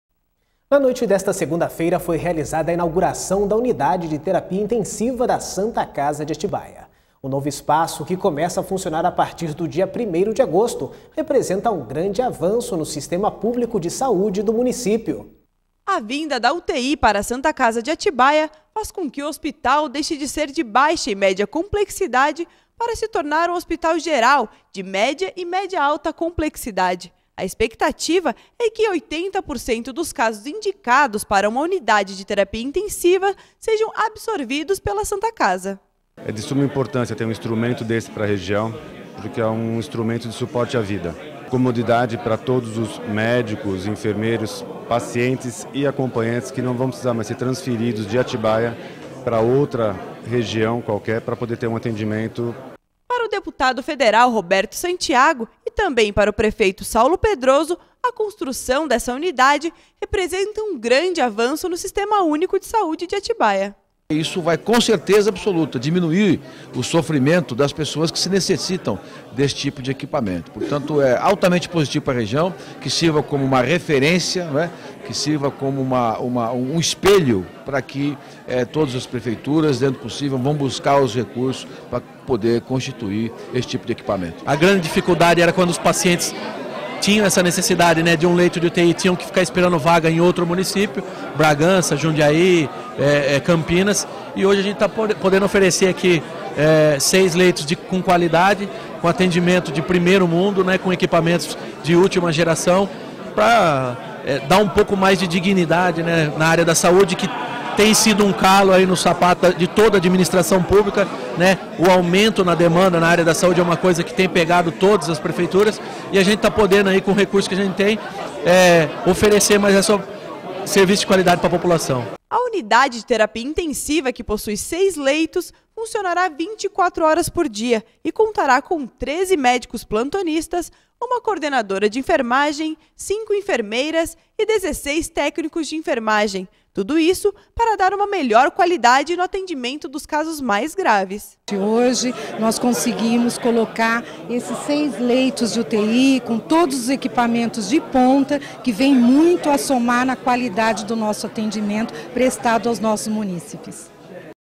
Inauguração UTI Sta Casa
Na noite desta segunda foi realizada a inauguração da Unidade de Terapia Intensiva da Santa Casa de Atibaia. O novo espaço, que começa a funcionar a partir do dia primeiro de agosto, representa um grande avanço no Sistema Público de Saúde do Município.